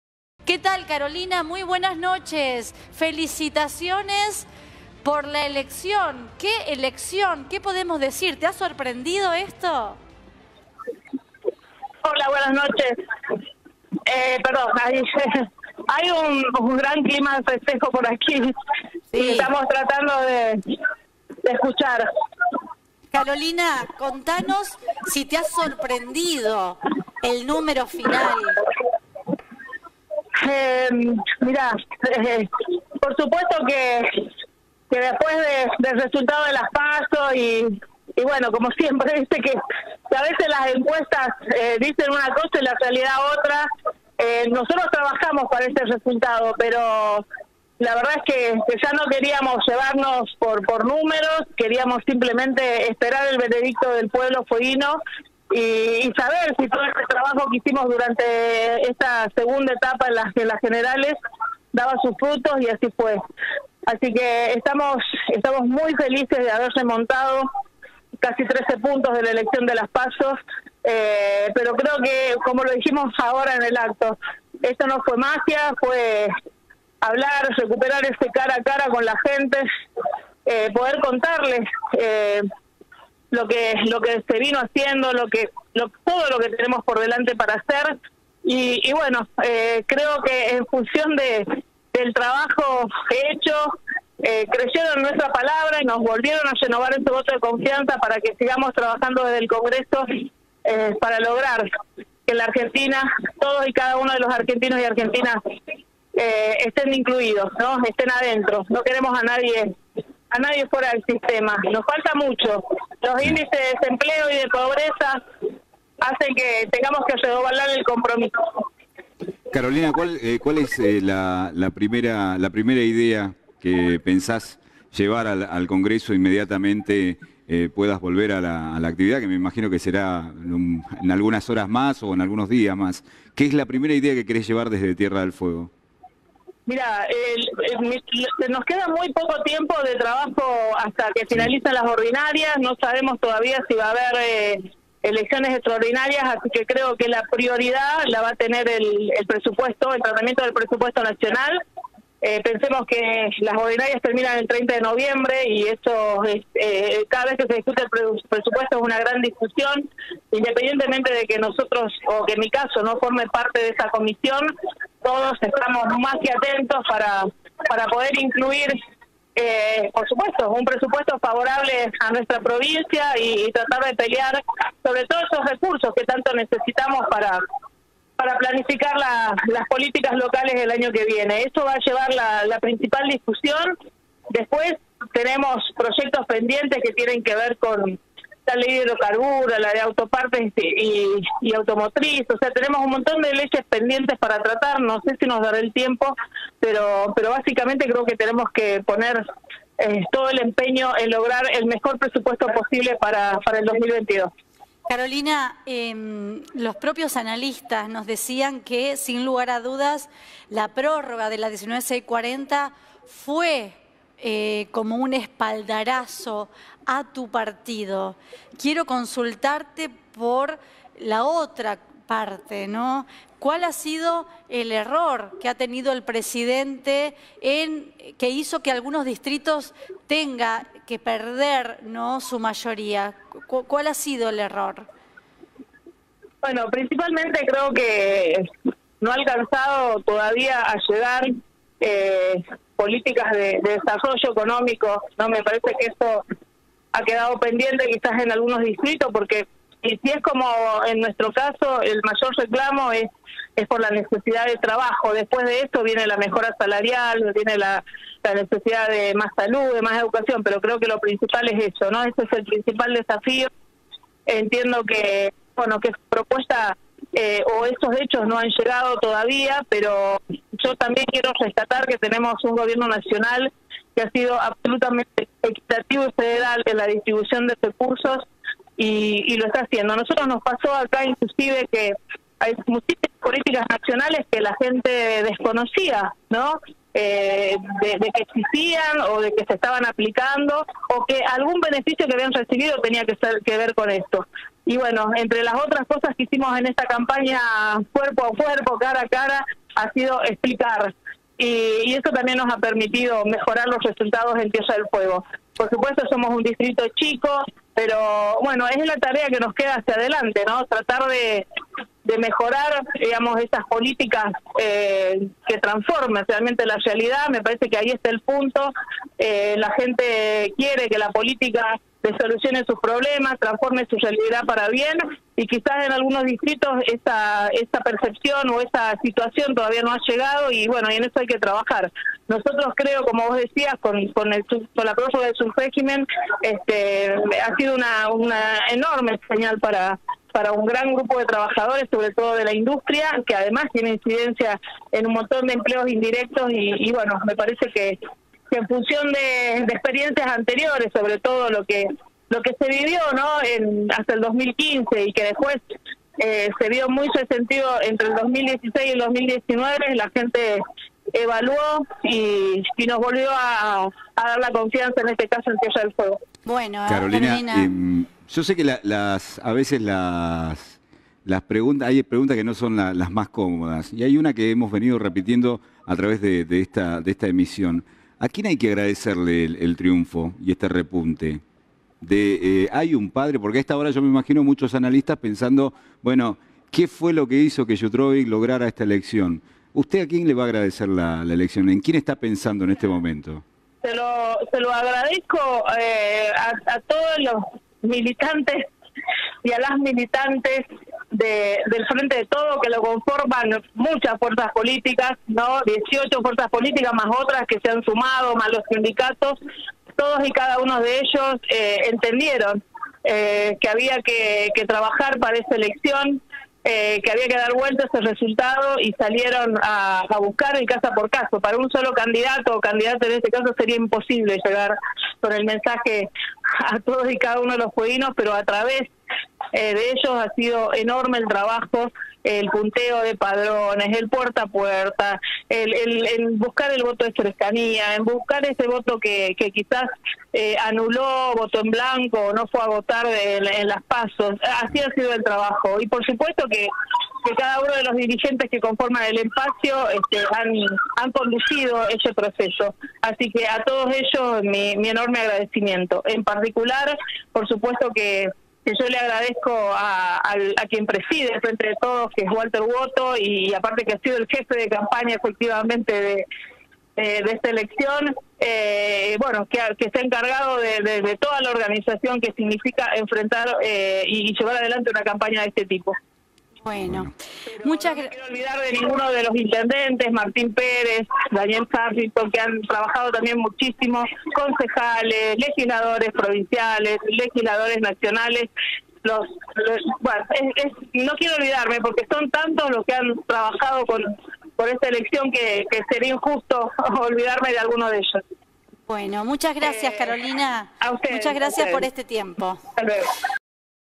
YUTROVIC: "ESTAMOS FELICES DE HABER REMONTADO LAS PASO"
La diputada Carolina Yutrovic retuvo su banca para Tierra del Fuego y analizó en I3N el triunfo con más de 10 puntos, "la tarea que nos queda hacía delante es mejorar las políticas que transformen la realidad".